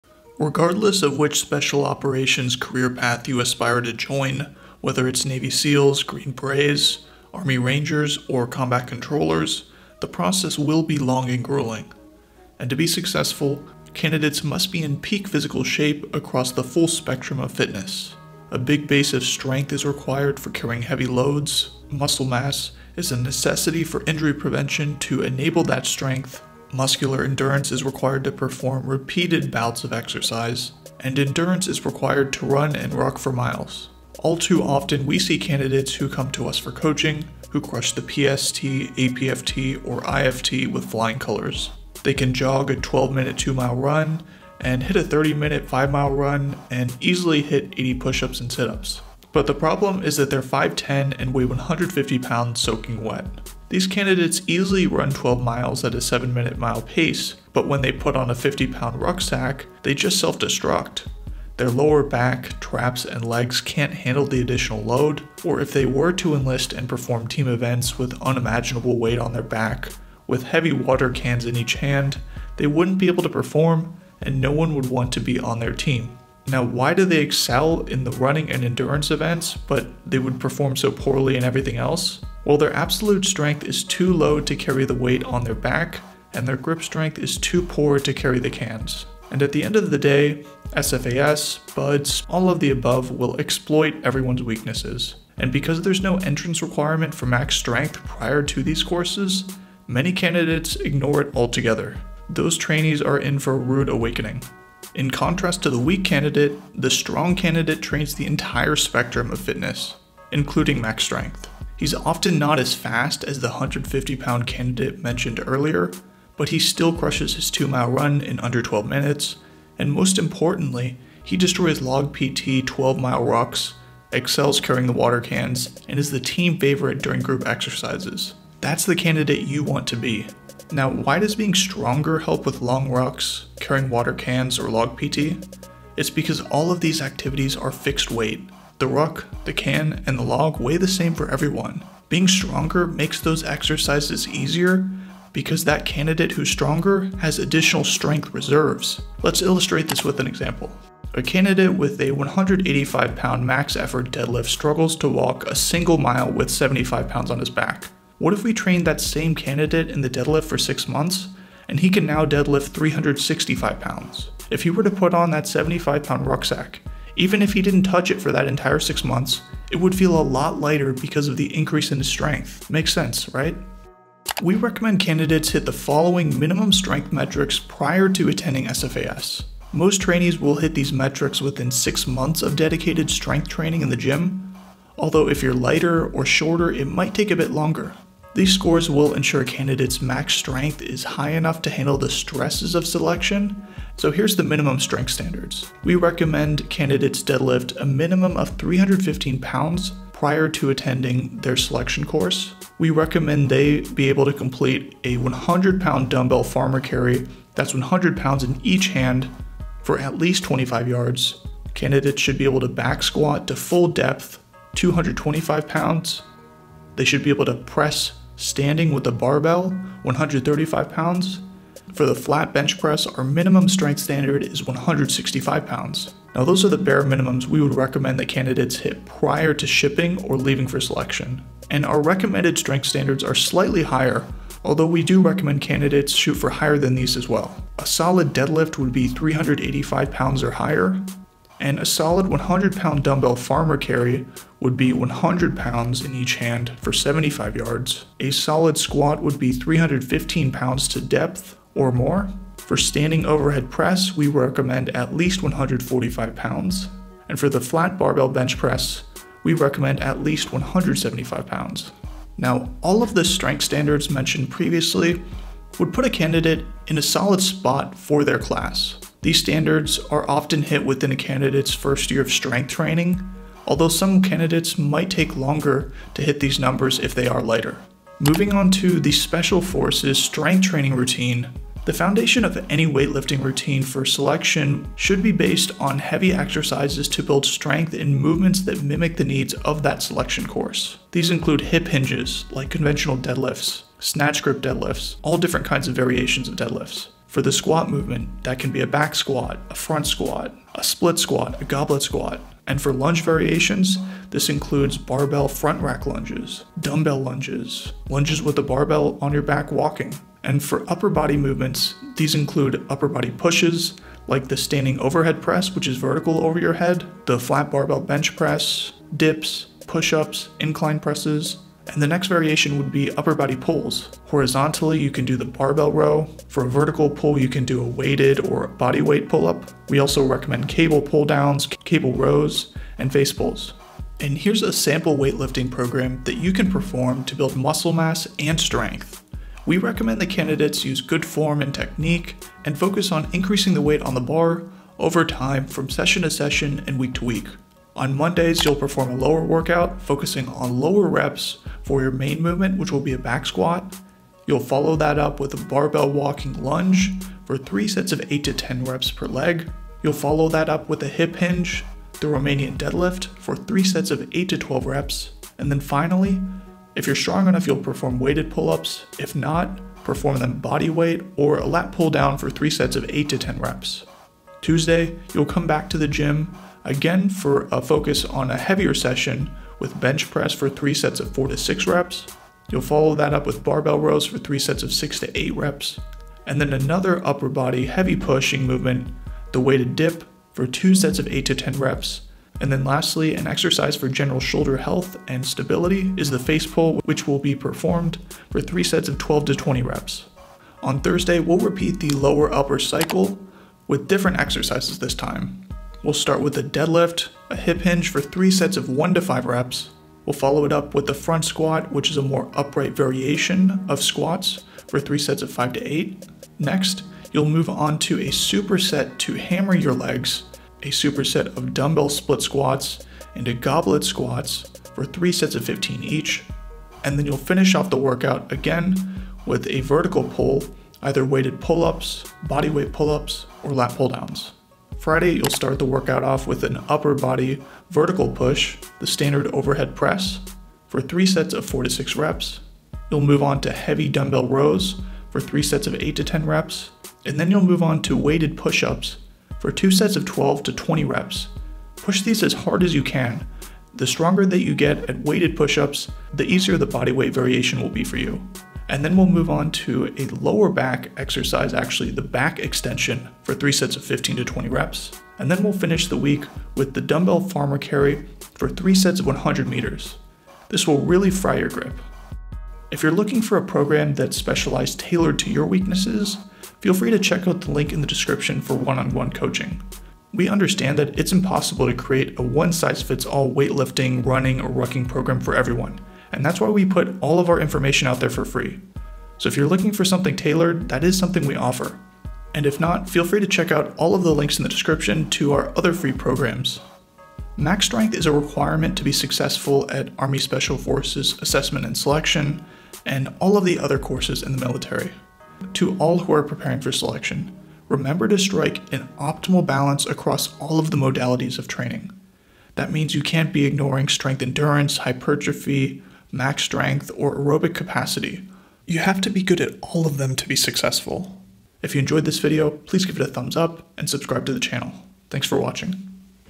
BEST WEIGHT LIFTING PROGRAM FOR SPECIAL FORCES | SEAL, GREEN BERET, RANGER, PJ TRAINING TIPS
Regardless of which special operations career path you aspire to join - whether it’s Navy SEALs, Green Berets, Army Rangers, or Air Force PJs, the process will be long and grueling. To be successful, candidates must be in peak physical shape across the full spectrum of fitness - a big base of strength for carrying heavy loads, muscle mass for injury prevention to enable that strength, muscular endurance to perform repeated bouts of exercise, and endurance to run and ruck for miles.

Video Chapters:
0:00 - Introduction
0:34 - The WEAK candidate
1:44 - The STRONG candidate
2:15 - The benefits of strength
2:53 - Strength standards for SOF
4:40 - Foundational exercises
5:40 - Strength training program for SOF
5:47 - Day 1 (Lower)
6:13 - Day 2 (Upper)
6:41 - Day 3 (Lower)
7:13 - Day 4 (Upper)
7:51 - Conclusion

Whether a candidate is training for BUD/S to become a Navy SEAL, or Air Force Special Warfare Candidate Course to become a Pararescueman (PJ), Combat Controller (CCT), Special Reconnaissance Operator (SR), Special Tactics Officer (STO), Combat Rescue Officer (CRO), or going to selection to become a Marine MARSOC Recon/Raider, or even preparing for the long walk to join CAG/DELTA, they will be surprised to learn these statistics.